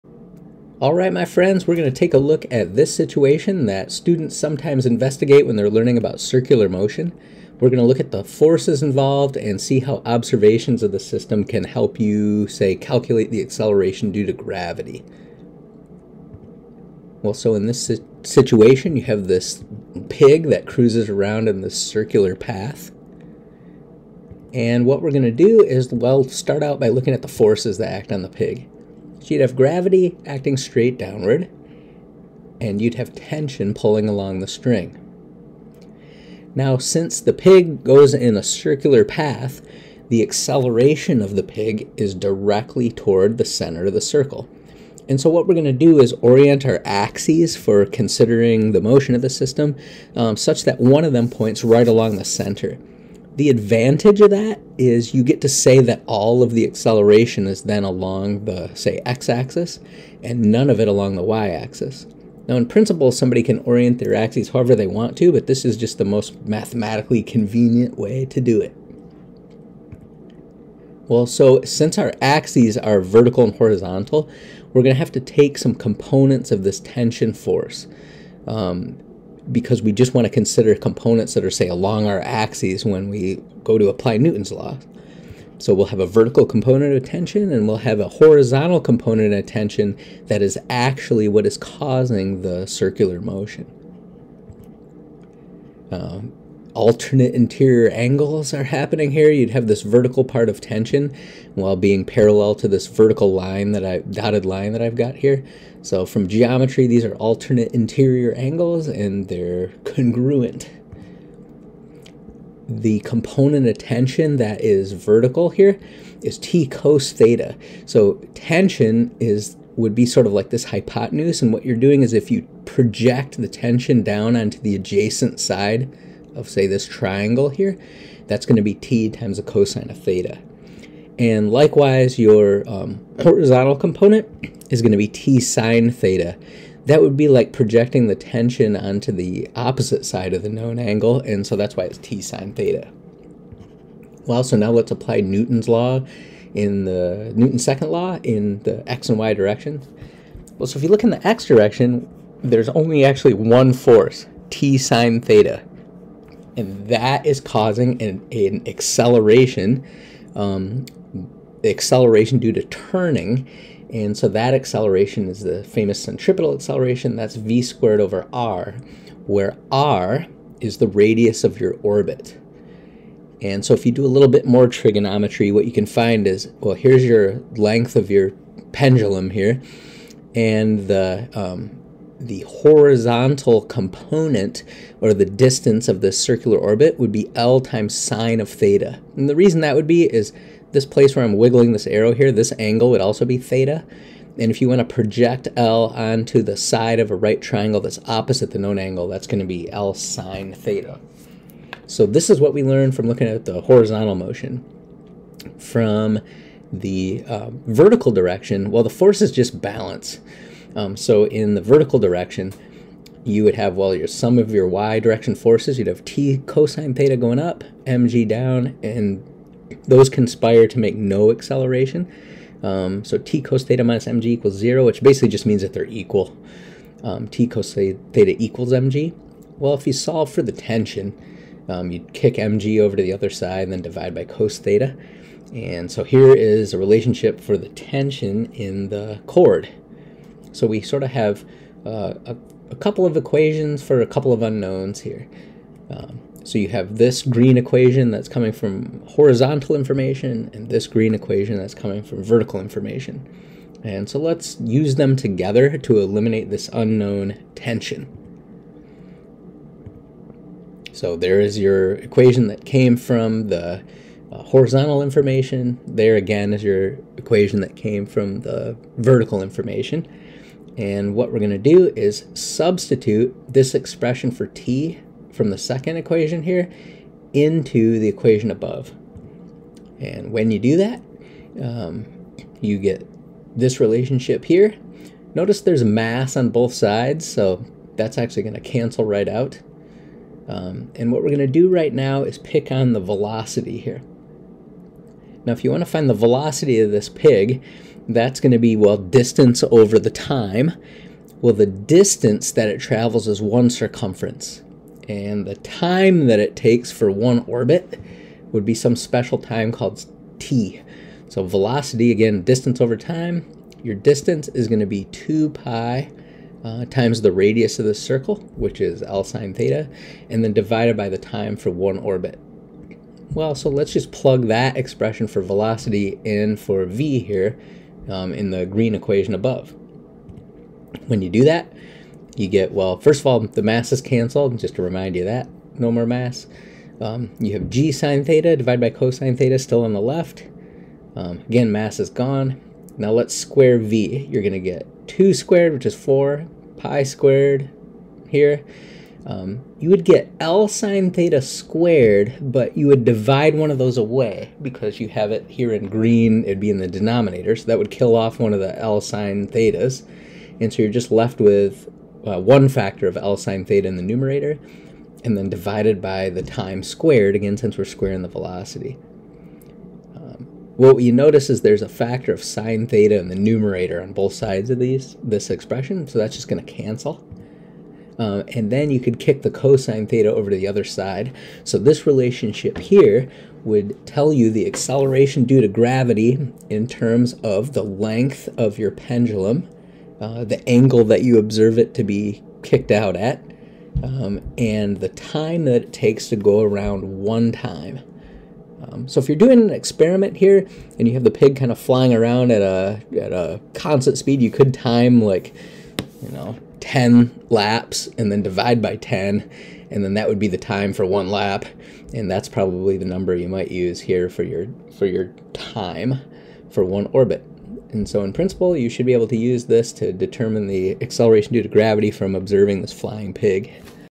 Flying Pig: Uniform Circular Motion and Centripetal Force
We look at the geometry of the popular flying pig experiment.  This is sometimes called a conical pendulum.  Students might encounter this experiment when studying uniform circular motion.  In this case, the horizontal component of a string tension provides the centripetal force and associated centripetal acceleration (v^2 / R).  The vertical component of tension is equal and opposite to the force of gravity (the weight) of the pendulum bob, or piggy.  We derive a scheme for calculating the acceleration due to gravity in terms of pendulum length (L), pendulum angle from vertical (theta), and the period of one orbit (t).